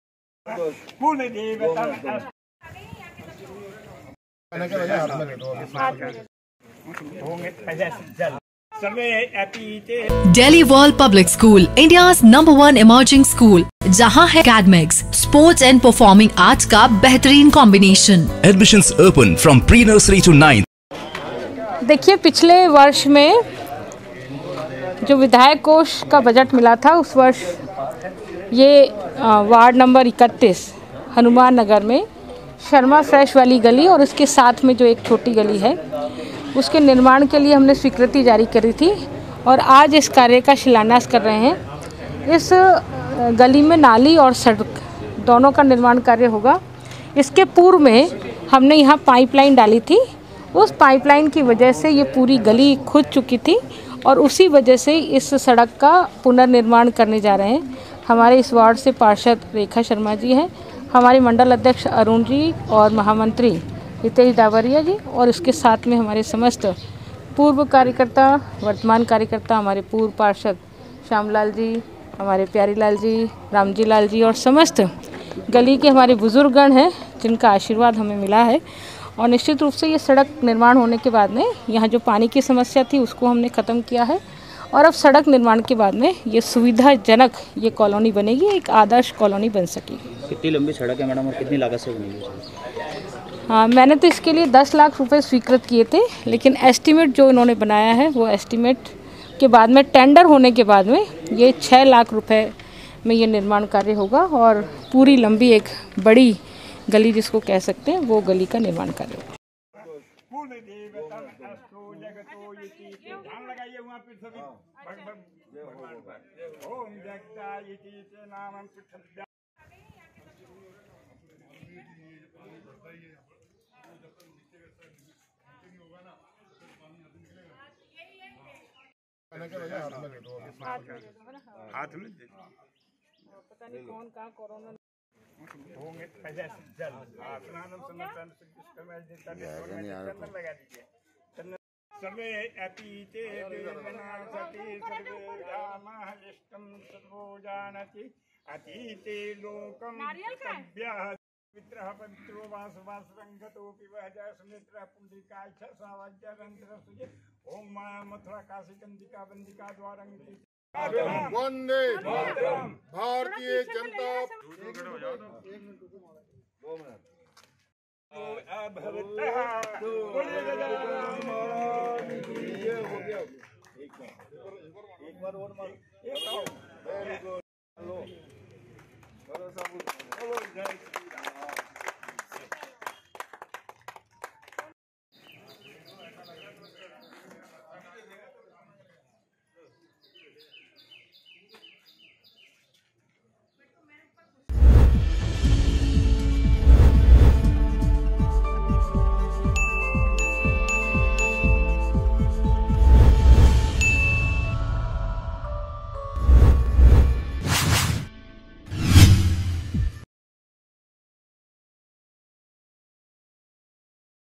Ajmer | दक्षिण विधायक अनिता भदेल ने सड़क निर्माण कार्य का किया शिलान्यास | MTTV INDIA
अजमेर दक्षिण विधायक अनिता भदेल ने सड़क निर्माण कार्य का किया शिलान्यास
वार्ड 31 की हनुमान नगर कॉलोनी में अब बनेगी सड़क और नाली
विधायक कोष से होगा निर्माण कार्य
इस दौरान विधायक अनिता भदेल को बुजुर्ग ने पीएम केयर फंड के लिए दिए 1100 रुपए